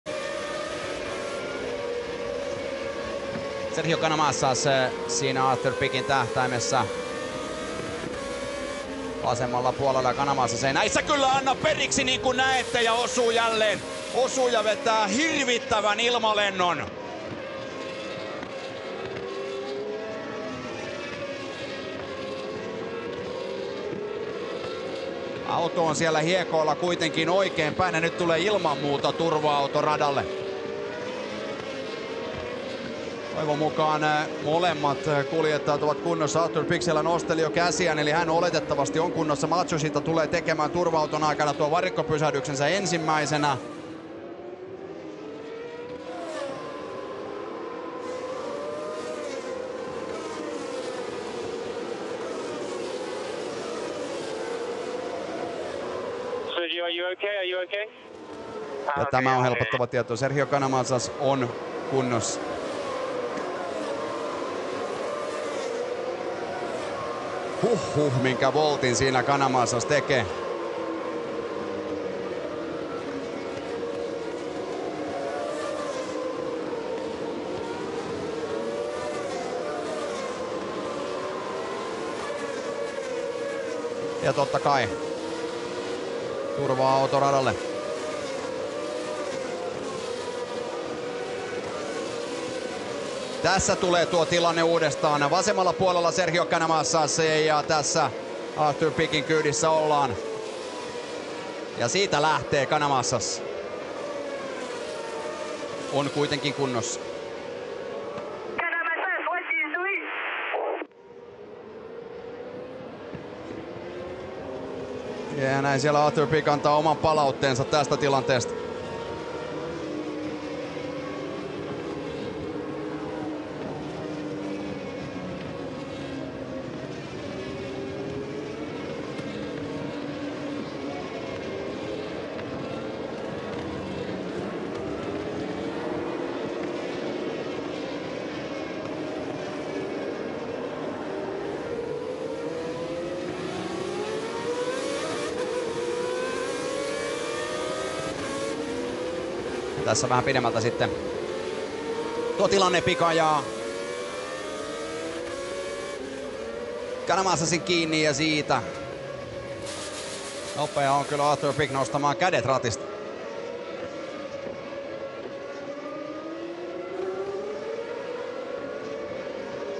Monza 2016 GP2 Race 1 Pic Canamasas (Slideshow)
Just a slideshow to bypass YouTube's Content ID -system
Selostus: Oskari Saari